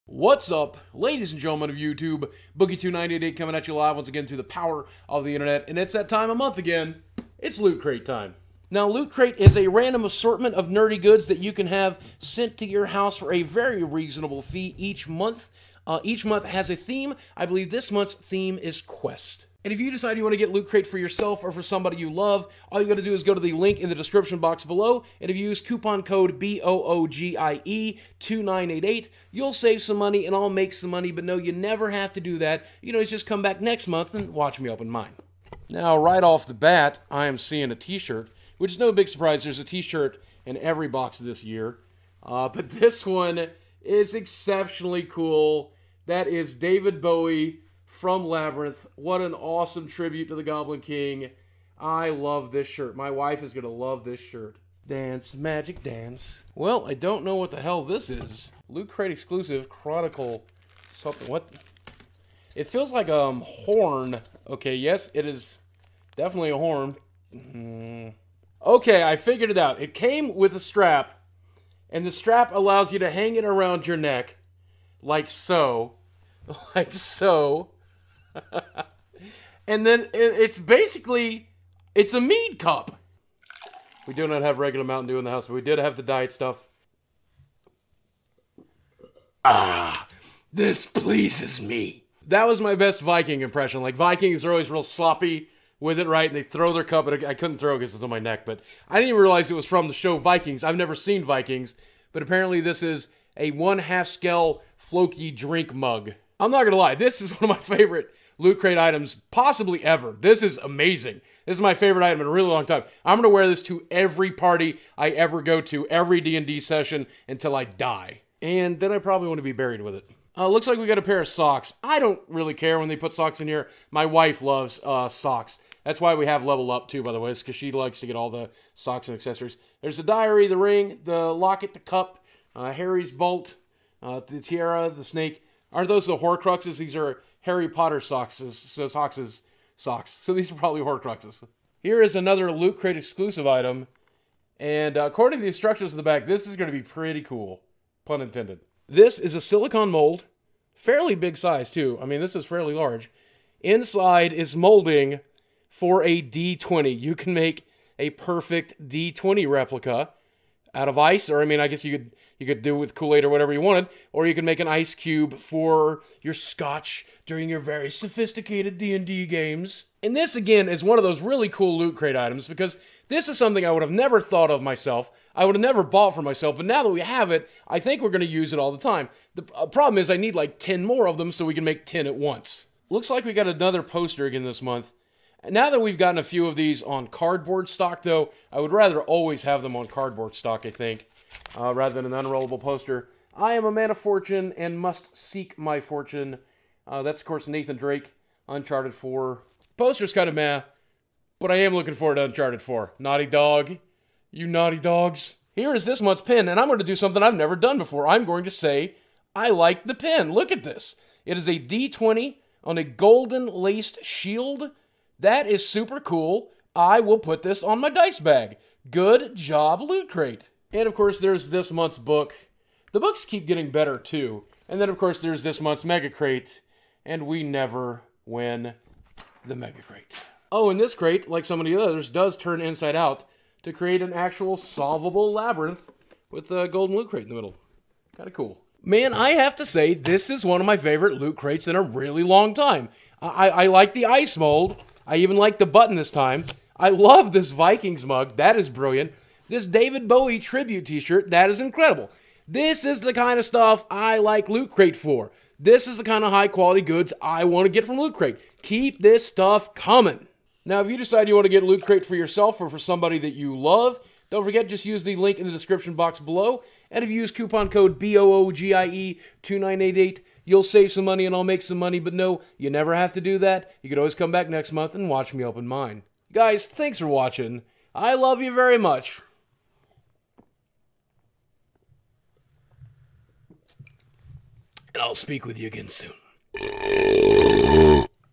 QUEST! Loot crate Unboxing April 2016!
Want lootcrate for yourself or someone you love? use this link

This month's loot crate for April 2016 is called 'quest' and has several items from several RPG oriented items.  There is a ice mold for a d20, there is a mug from the show vikings, a shirt featuring david bowie from Labrinth, and a poster from Uncharted 4.  Also some harry potter socks!

since i do stand to make money from this video if you choose to purchase loot crate this is technically a sponsored video!